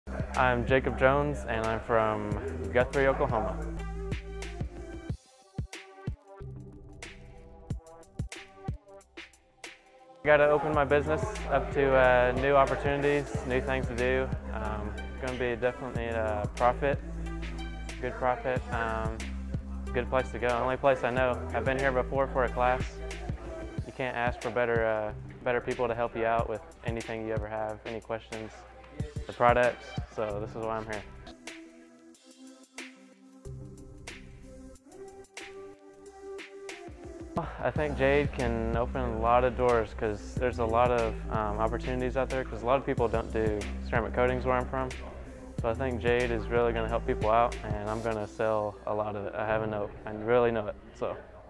Detail King Student Review- Jacob Jones Ceramic Coating Training June 2018
Check out what our former student, Jacob Jones, has to say about his experience at one of Detail King's Auto Detailing Training Seminars!

-To learn more information about our Ceramic Coating Training Programs!